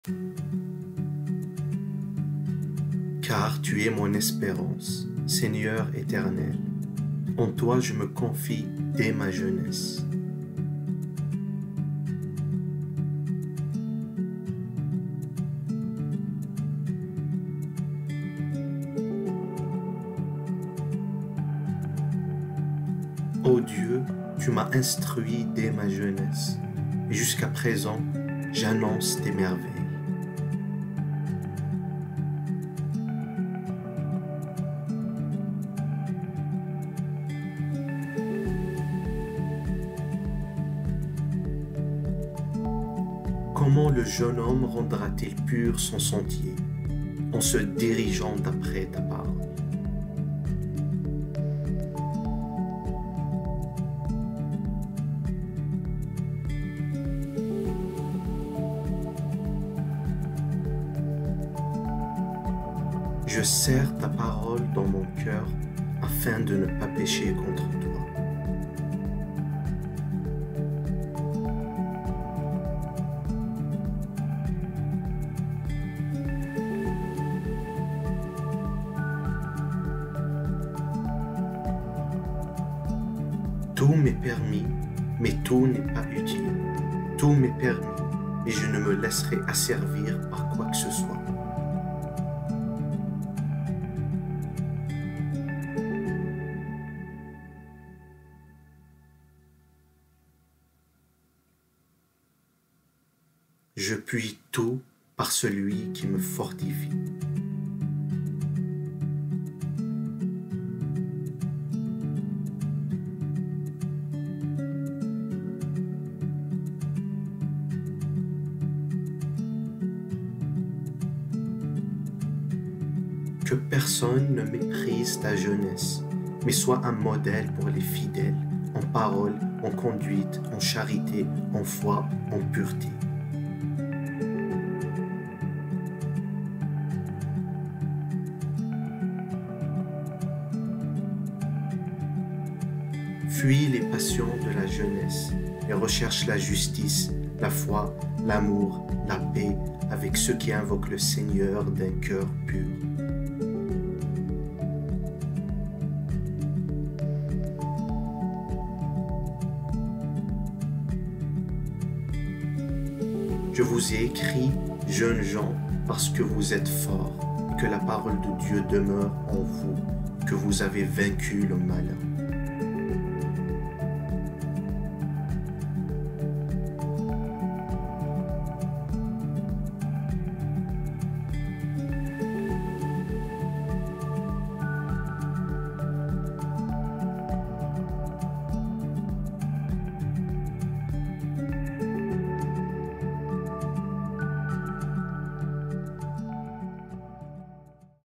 Versets bibliques pour les jeunes
Être jeune peut être une période de vie remplie de défis. Les passions et les désirs peuvent souvent sembler incontrôlables et il peut être difficile de savoir comment les gérer. Dans cette vidéo, découvrez comment la force de Dieu peut vous aider à surmonter ces obstacles. À travers des versets bibliques édifiants, vous apprendrez comment la parole peut vous donner la force de résister aux tentations et de suivre la voie que Dieu a prévue pour vous.